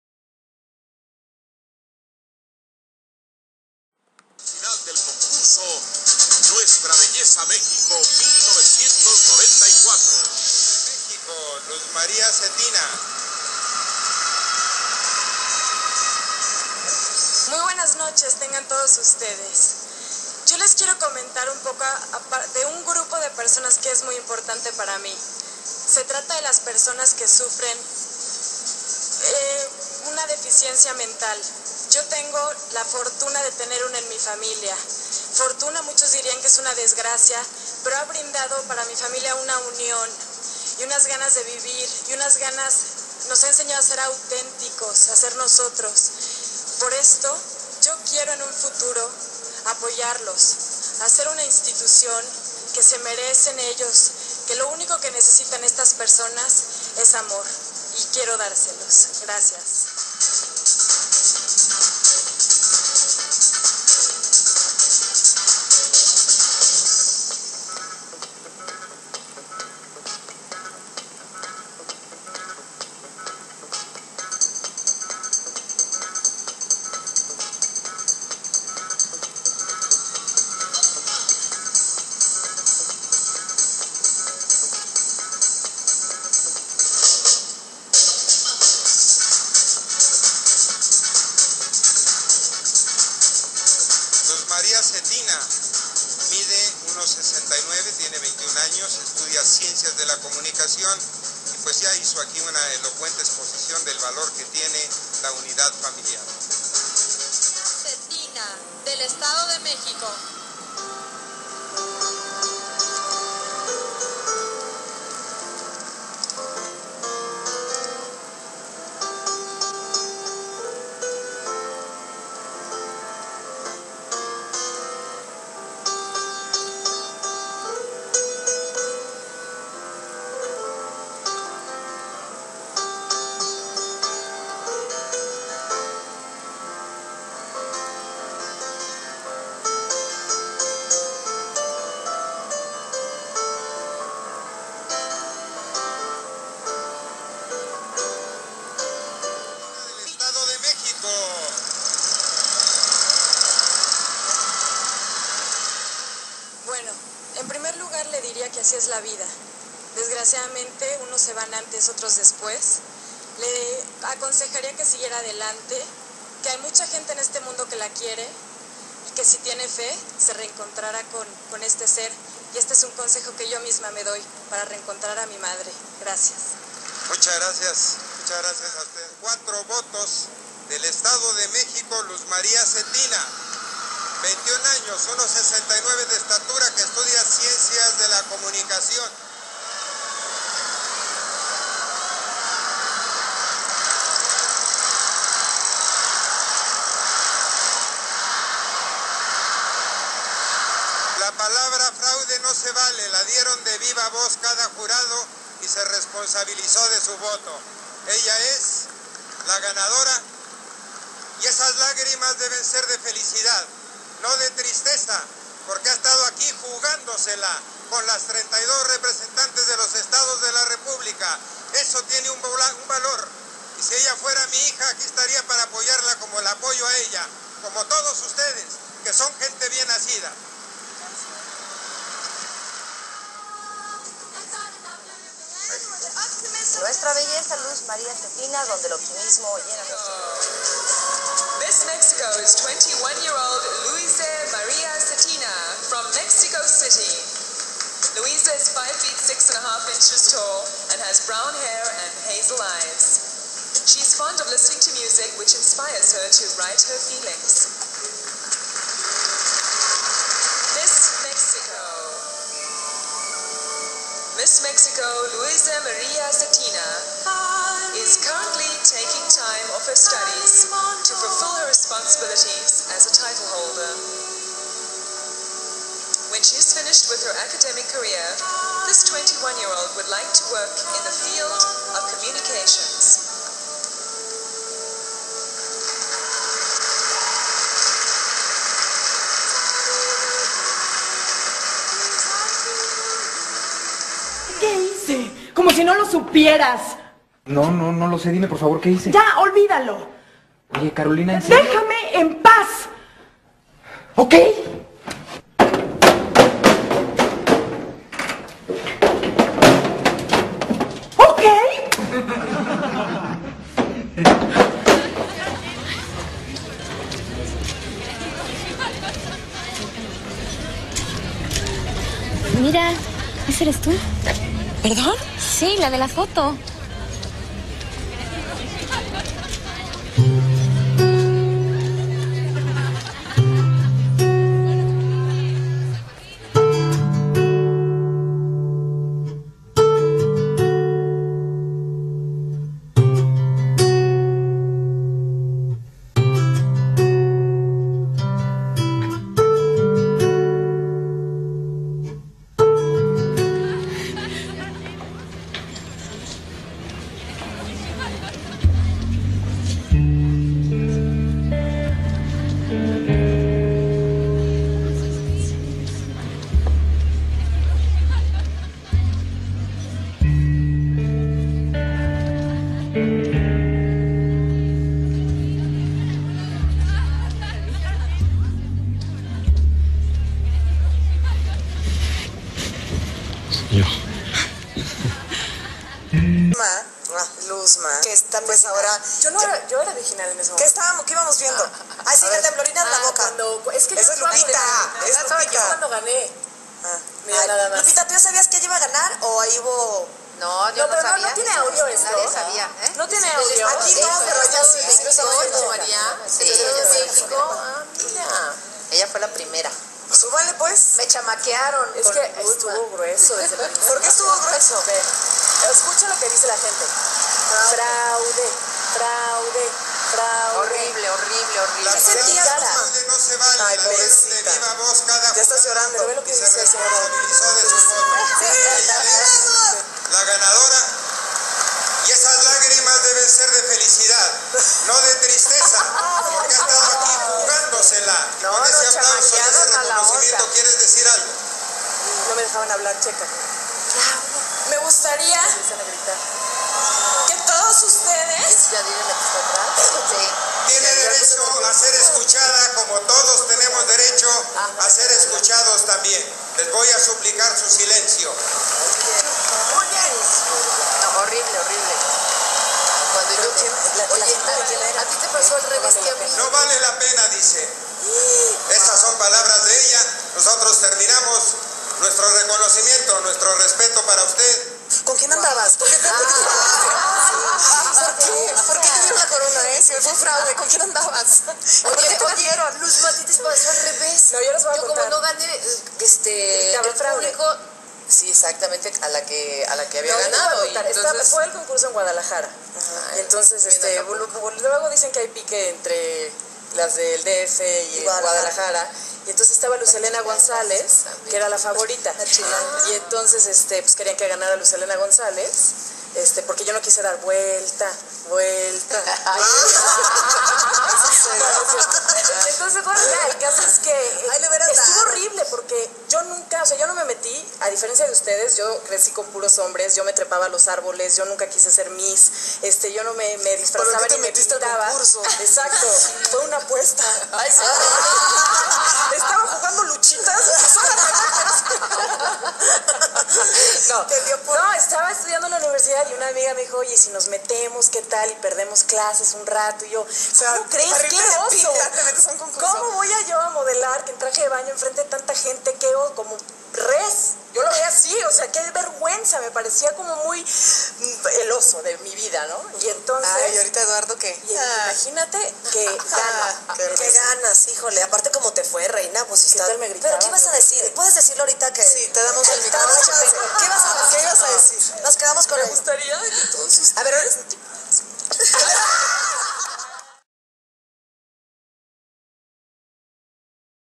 Luz Maria Zetina - Nueva belleza de Mexico 1994
Recuerdos maravillosos del año 1994 cuando Luz Maria Zetina con 21 años de edad se coronó como Nuestra Belleza México.